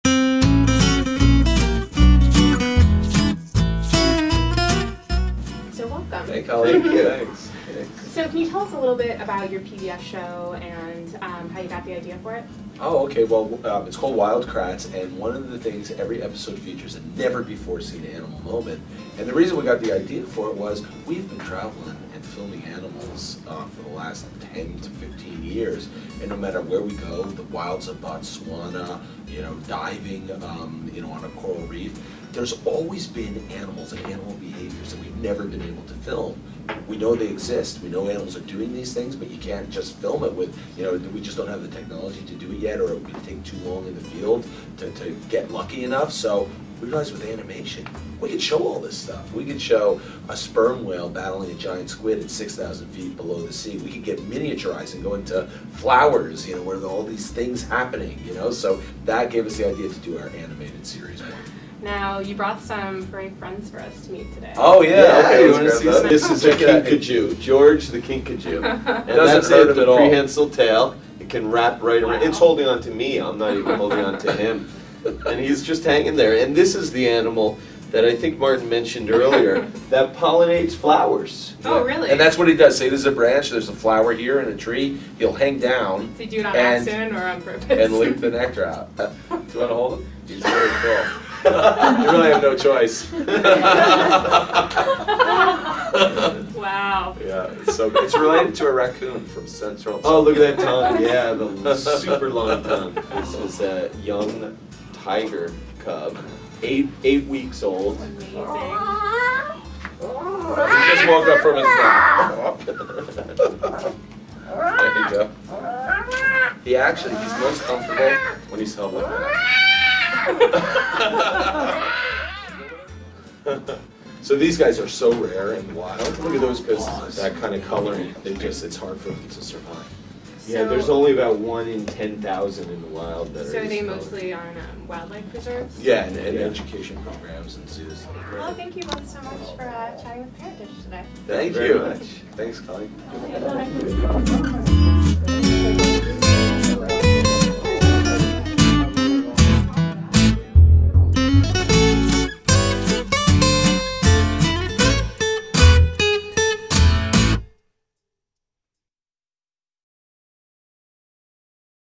'Wild Kratts' Get Frisky With ParentDish
When the Kratt brothers came to visit ParentDish, things got a little wild. Chris and Martin Kratt star in PBS Kids' "Wild Kratts," which combines animation and live action to teach kids about the amazing world of animals. The Kratts brought along two of their furry (and fiesty) friends: a kinkajou and a white Bengal tiger cub.